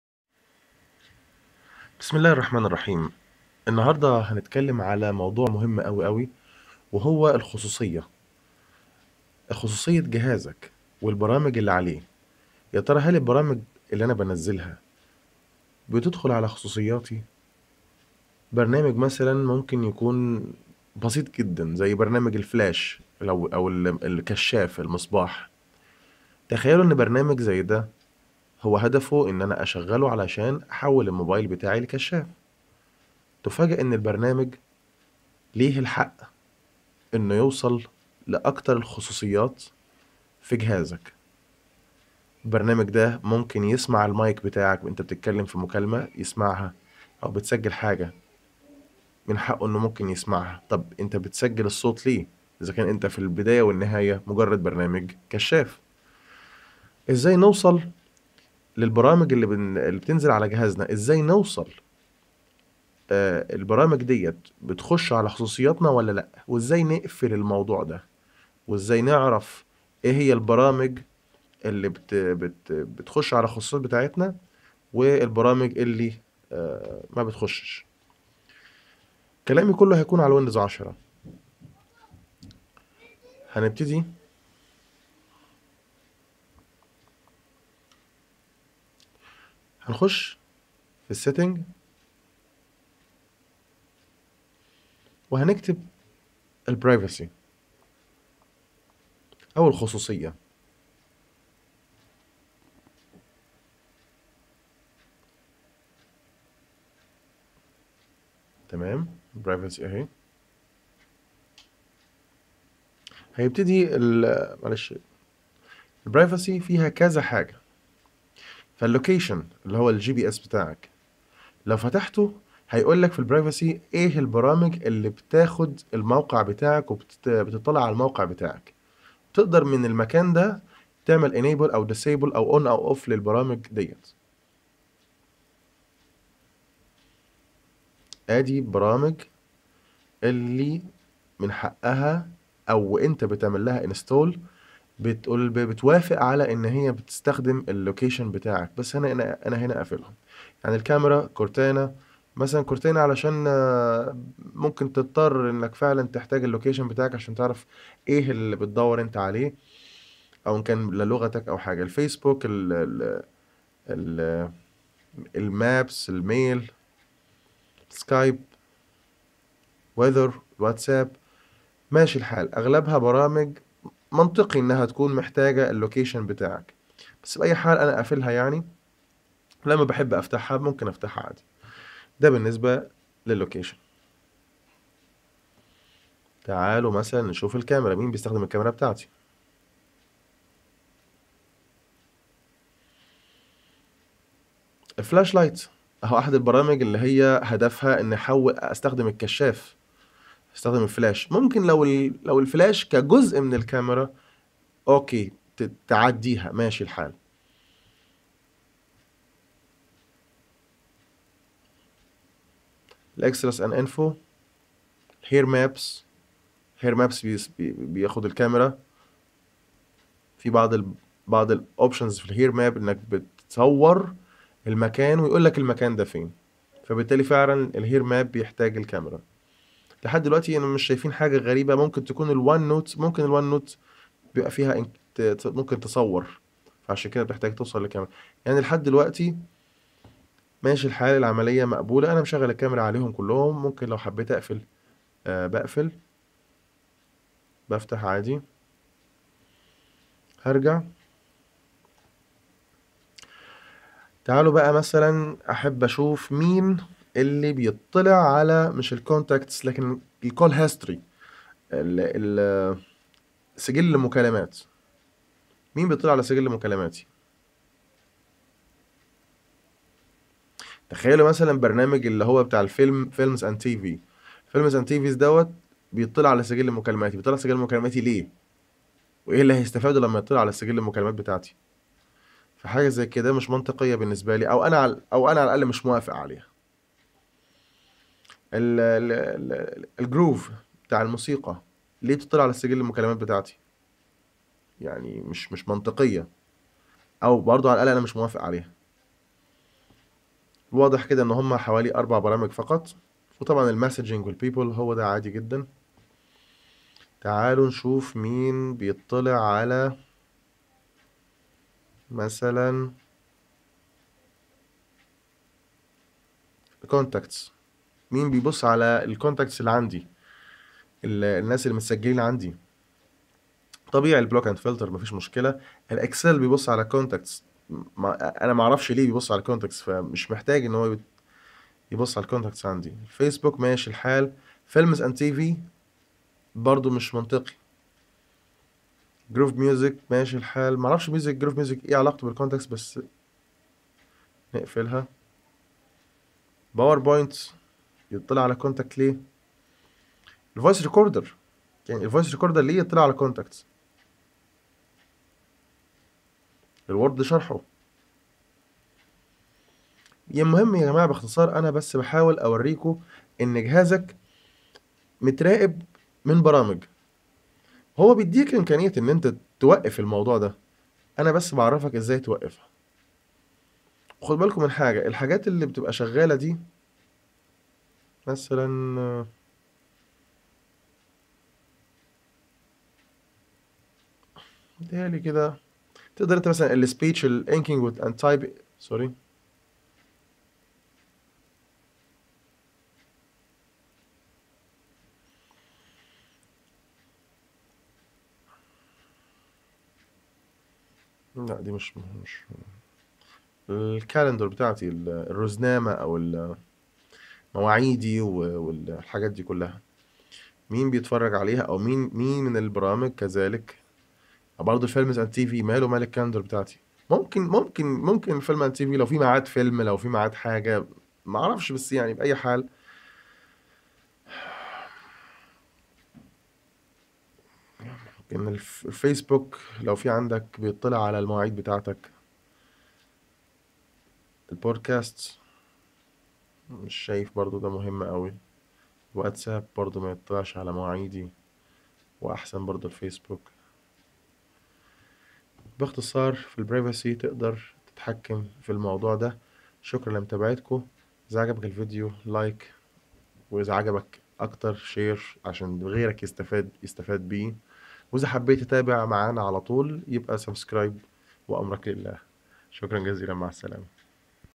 إحذر ويندوز 10 التجسس على خصوصياتك Windows phone 10
كيفية التعامل مع البرامج المتطفلة على خصوصياتك و منعها من التطفل مع ضمان تشغيلها
و التحكم في ما تستطيع الوصول له بإذنك أنت فقط
هامة جداً عن خصوصياتك